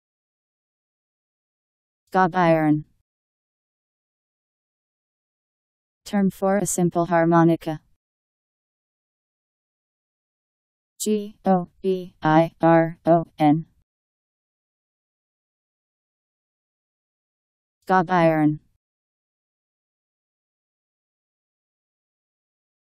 Gob iron Meaning
Video shows what gob iron means. Term for a simple harmonica.  Gob iron Meaning. How to pronounce, definition audio dictionary. How to say gob iron. Powered by MaryTTS, Wiktionary